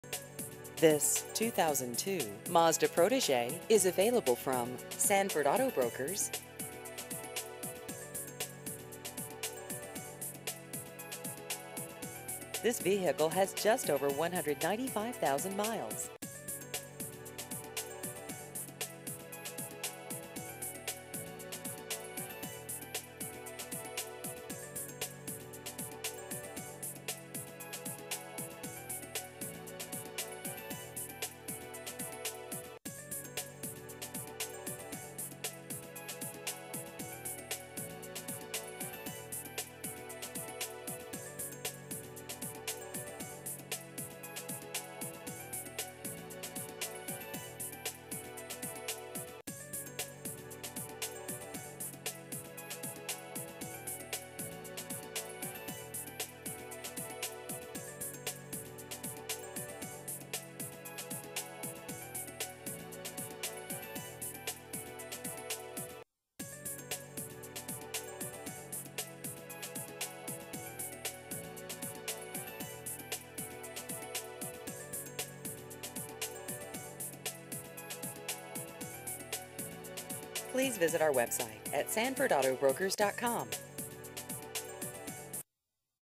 2002 Mazda Protege Used Cars Sanford NC
This 2002 Mazda Protege is available from Sanford Auto Brokers.